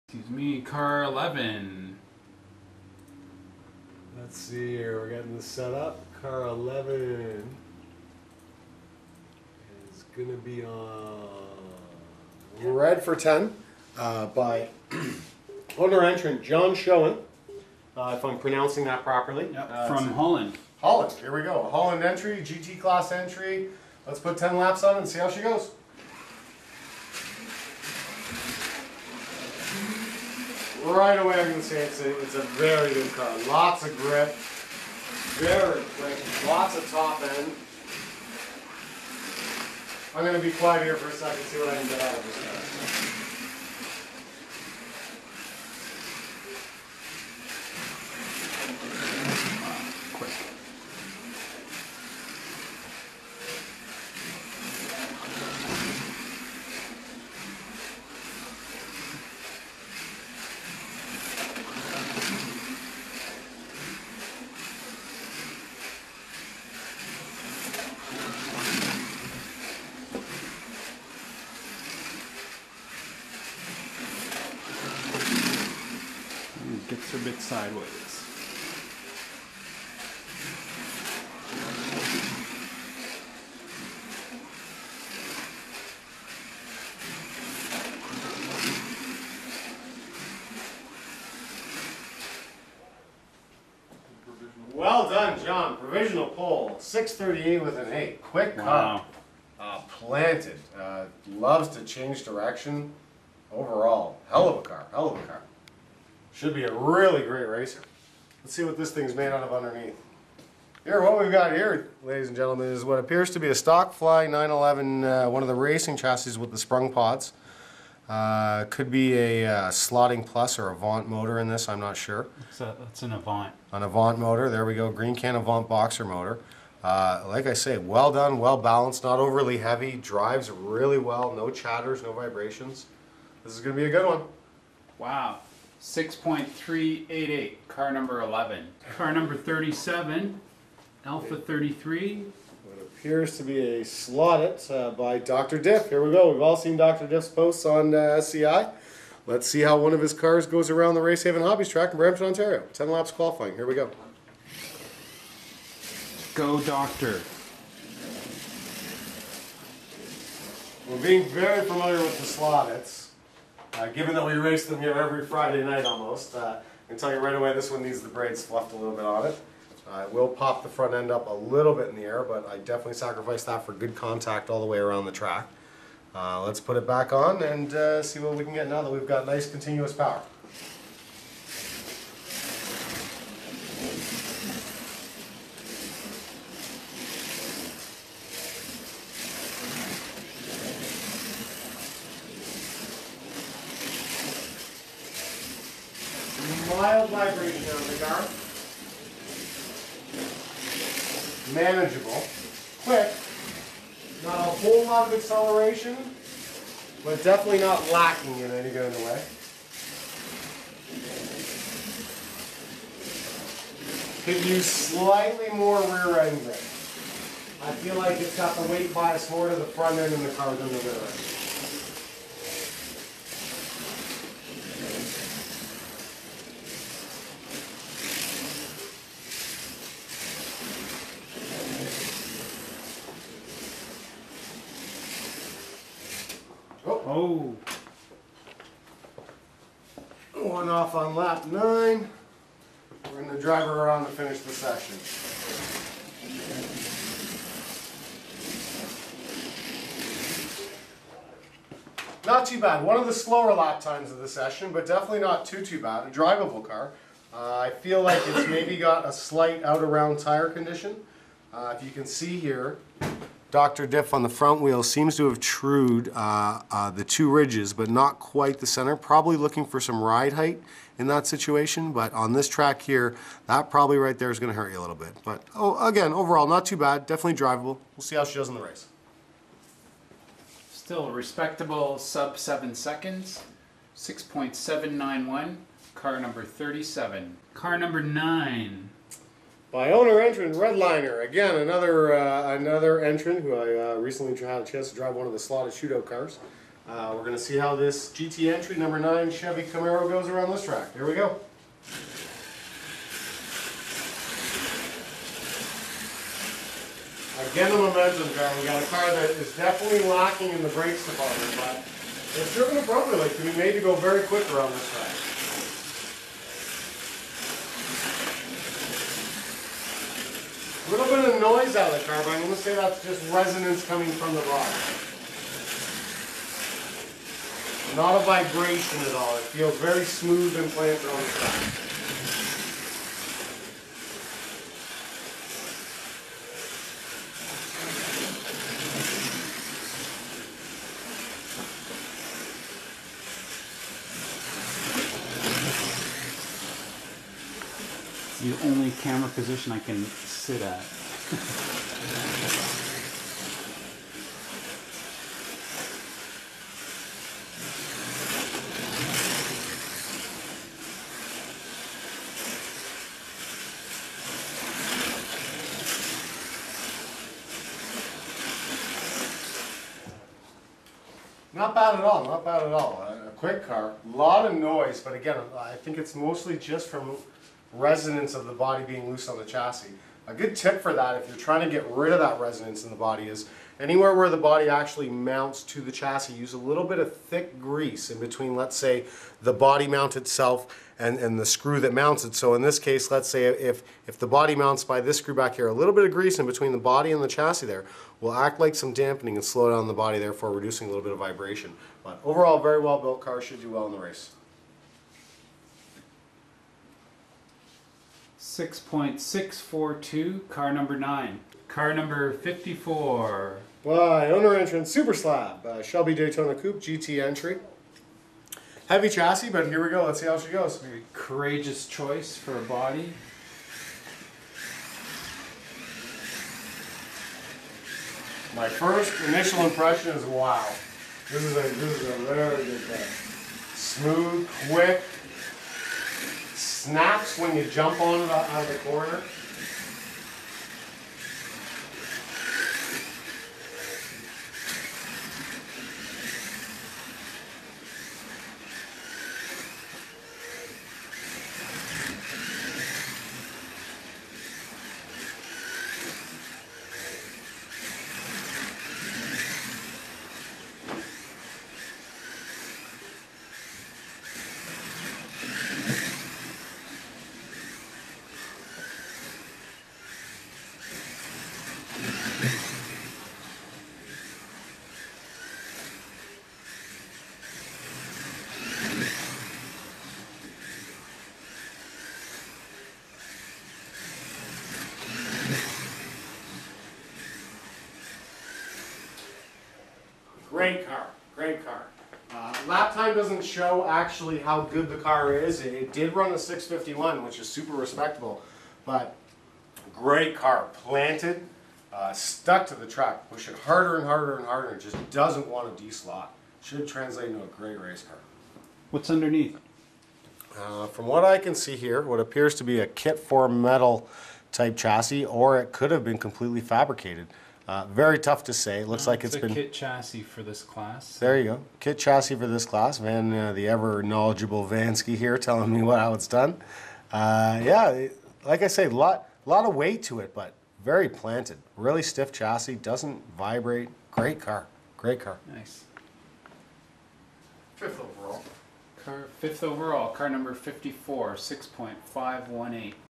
CanAm 2010 Round 8 Part 8
Qualifying for round 8 of the CanAm Proxy Race featuring GT#11/Joh Schoen, P#37/Dr. Diff, GT#9/redlynr and GT#54/SupeerSlab. or drop by the store to learn more about model car racing.